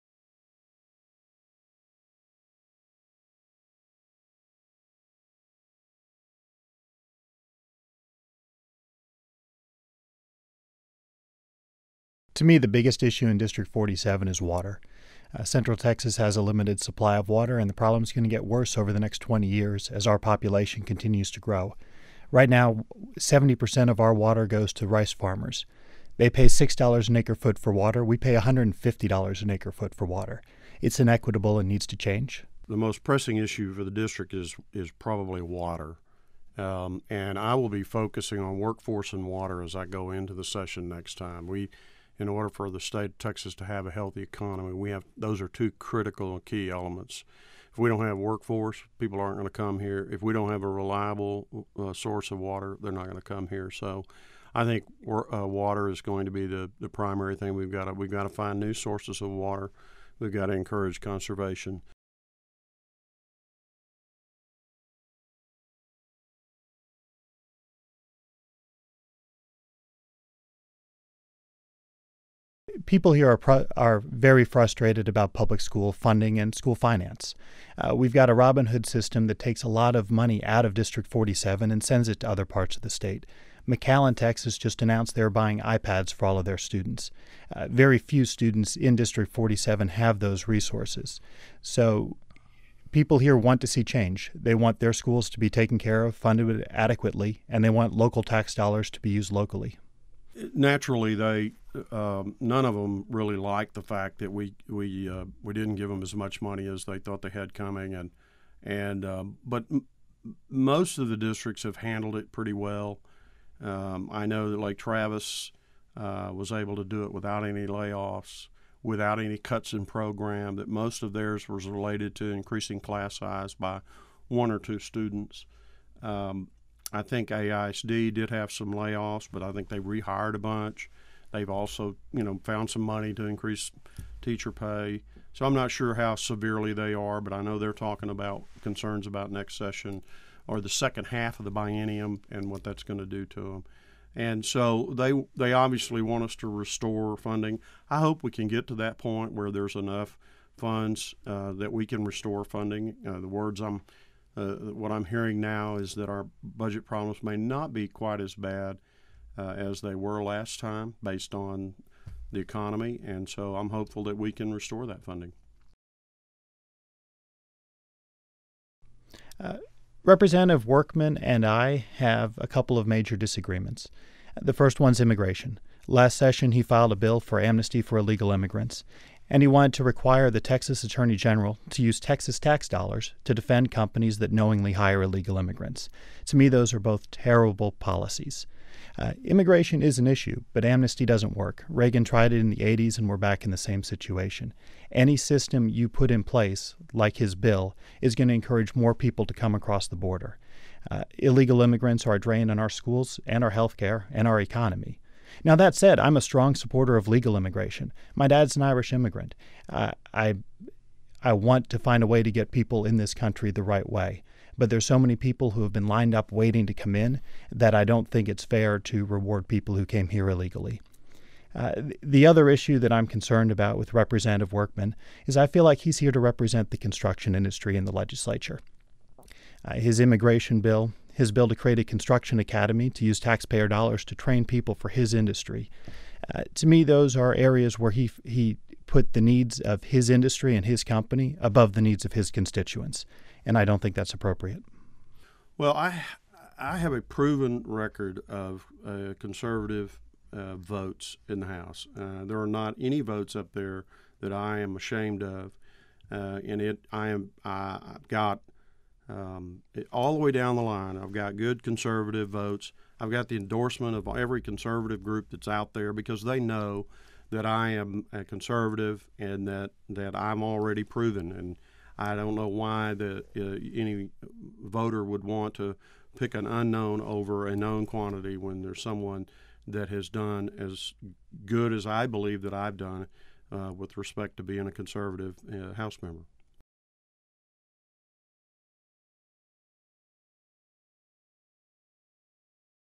House District 47 Republican Candidates
Texas State Representative Paul Workman and his GOP primary challenger Ryan Downton talk about the issues in House District 47.